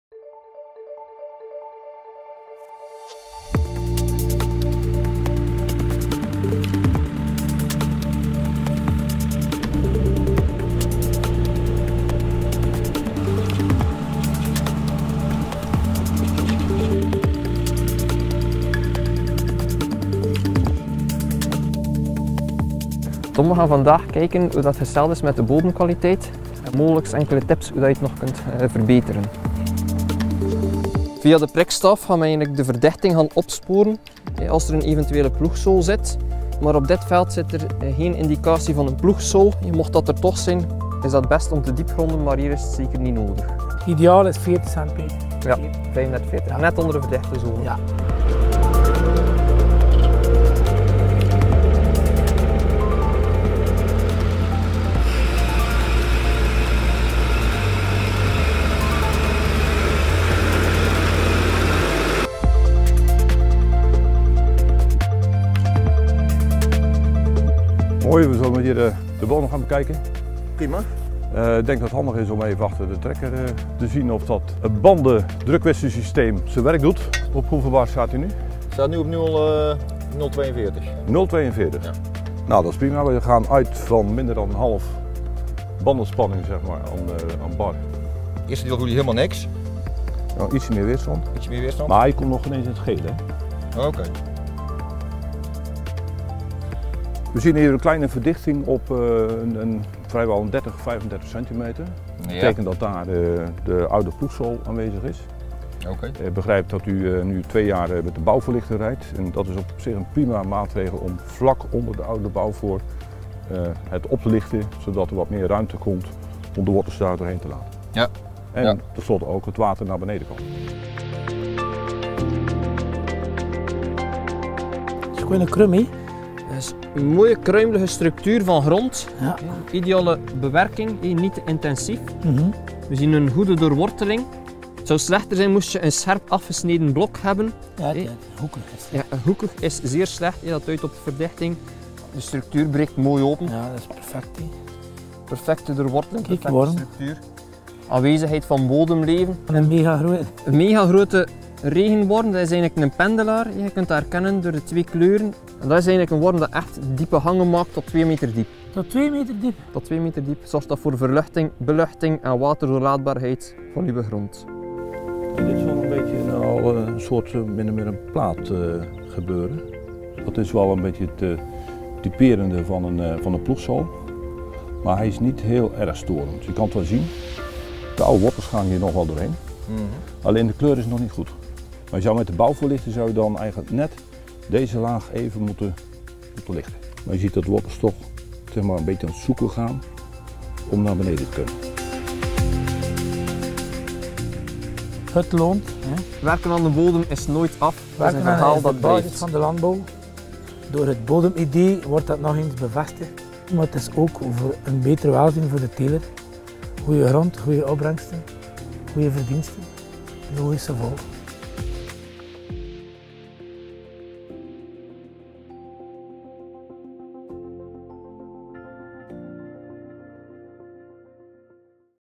Kijk in en naar je bodem
De bodem is de basis van een gezond gewas en een goede opbrengst. Om de fysische en biologische bodemkwaliteit te evalueren, kan je als landbouwer zelf aan de slag. Met een prikstok kan je verdichting opsporen, en via een spadesteek kan je de bodemstructuur beoordelen. Een kruimelige kluit aarde en regenwormen vormen een goed teken. Vooral de pendelaar is erg gewenst. Die regenworm herken je aan zijn platte kop tijdens het bewegen en aan zijn donkerroze bovenkant met een bleke onderkant. Hij legt grote, verticale afstanden af en zorgt zo voor een luchtige bodem.